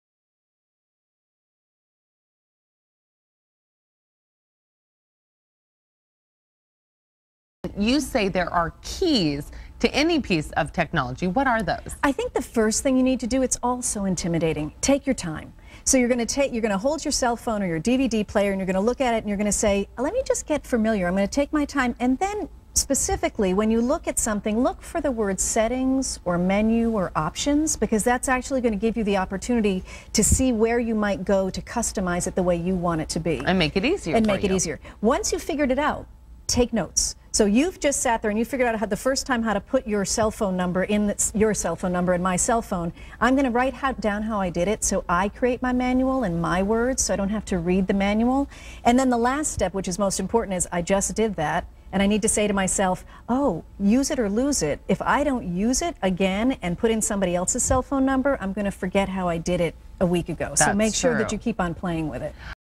Keys to Technology
Abby Stokes, author of "Is This Thing On?" A Computer Handbook for Late Bloomers, Technophobes, and the Kicking & Screaming, gives you the keys to unlocking technology whether it;s your computer, your DVD player, or your cell phone.